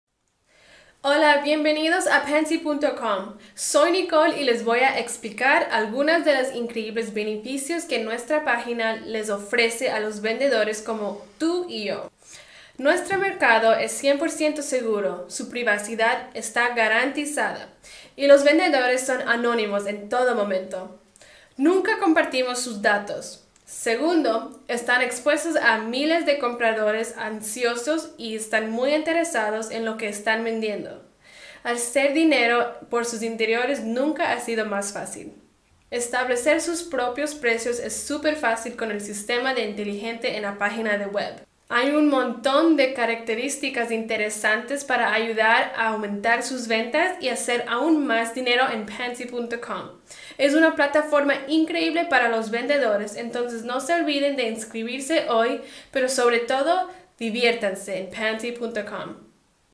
Panty.com: ventajas para las vendedoras - Panty.com
Si quieres vender tus bragas usadas en Panty.com aquí te cuento cómo hacerlo. Panty.com es seguro, anónimo y fácil de usar.